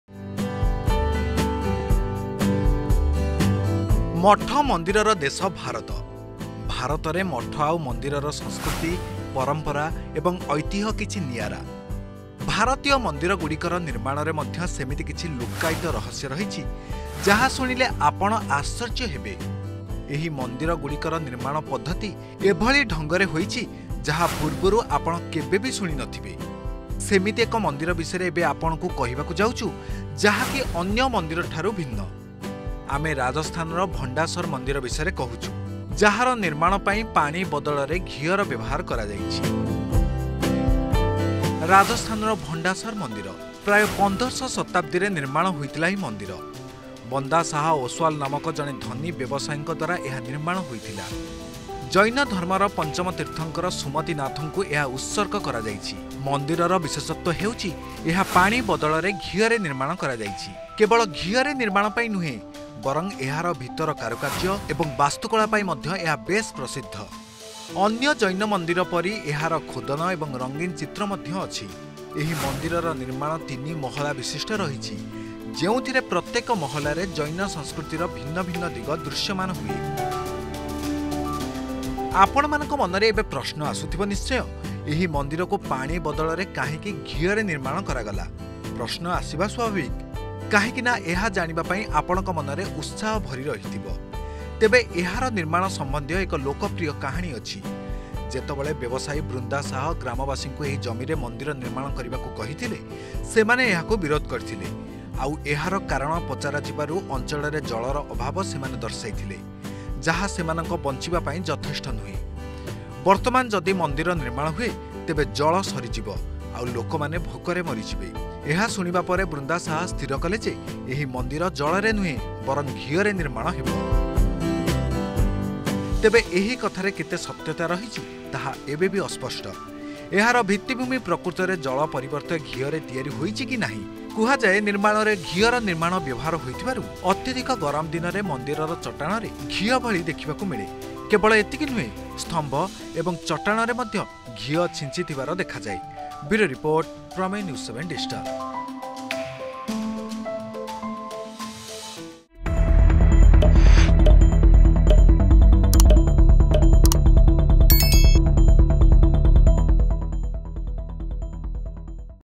Bhandasar Jain Temple |କାହିଁକି ଘିଅରେ ତିଆରି ହୋଇଛି 'ଭଣ୍ଡାସର ମନ୍ଦିର', କ'ଣ ରହିଛି ଏହା ପଛର ରହସ୍ୟ ?|Prameya
Bhandasar Jain Temple, Rajasthan : The Temple Made of Ghee
କାହିଁକି ଘିଅରେ ତିଆରି ହୋଇଛି 'ଭଣ୍ଡାସର ମନ୍ଦିର', କ'ଣ ରହିଛି ଏହା ପଛର ରହସ୍ୟ ?